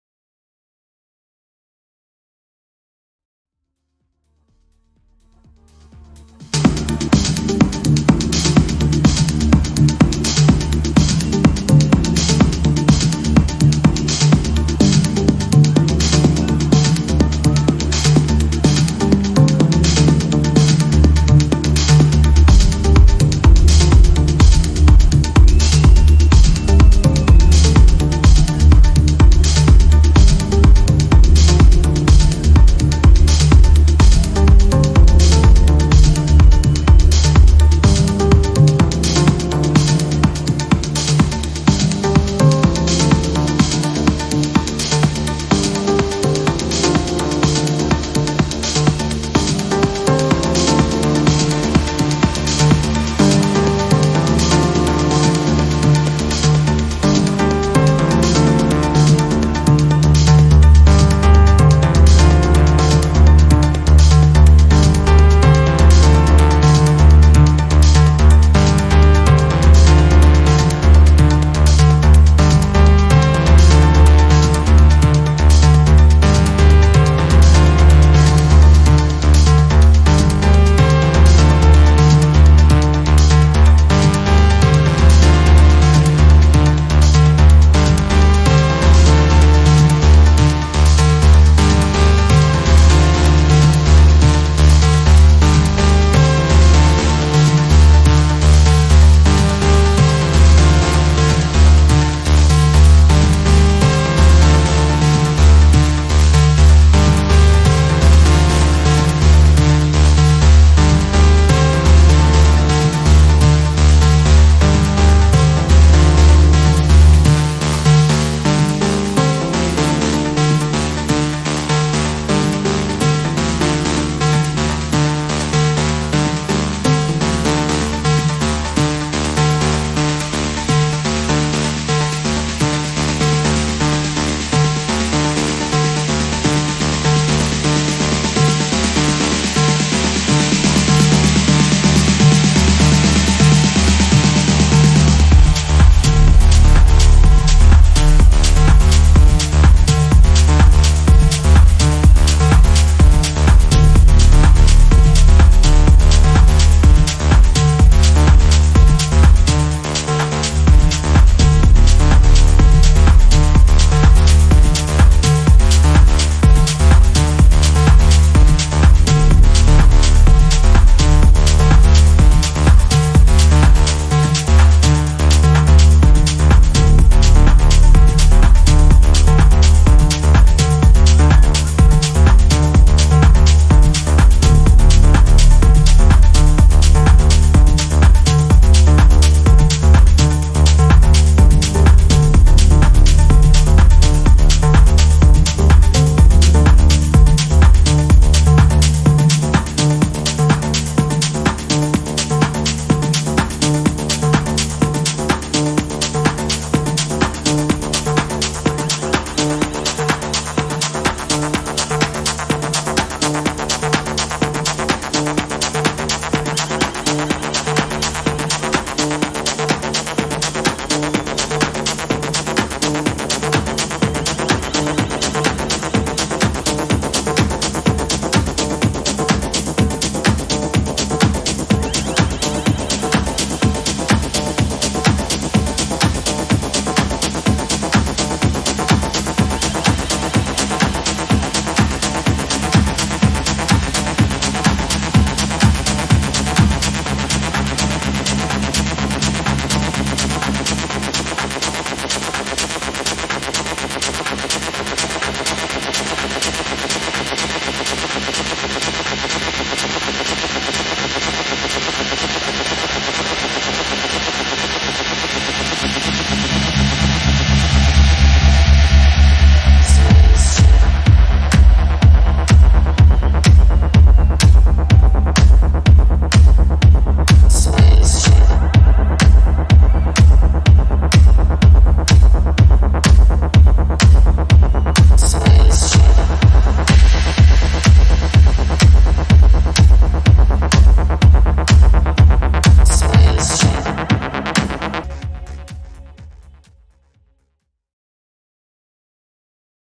🎧EXIT 2019 Part4🎵Adriatique Live📀[No Copyright]🎶
EXIT 2019 | Adriatique Live @ mts Dance Arena de ExitFestival está posteada bajo una  licencia Creative Commons.